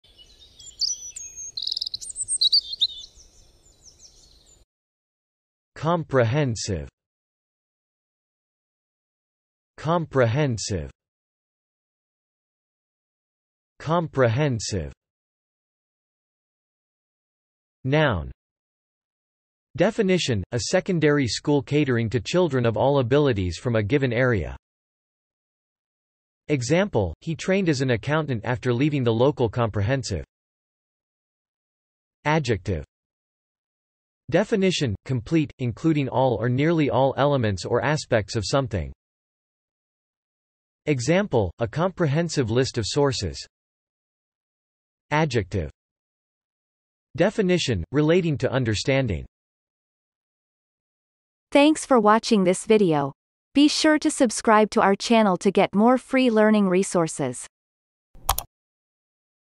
How to pronounce comprehensive - Vocab Today
The pronunciation of the word comprehensive in Amercian accent is demonstrated in this video. If the word is from another language, such as brand name, it will be pronounced in its original language. Based on the word frequency, this is of 2 stars difficulty.

The word comprehensive has the following meanings:
1. (adjective)
Definition : Complete; including all or nearly all elements or aspects of something.
Example : a comprehensive list of sources
2. (adjective)
Definition : Relating to understanding.

3. (noun)
Definition : A secondary school catering to children of all abilities from a given area.
Example : he trained as an accountant after leaving the local comprehensive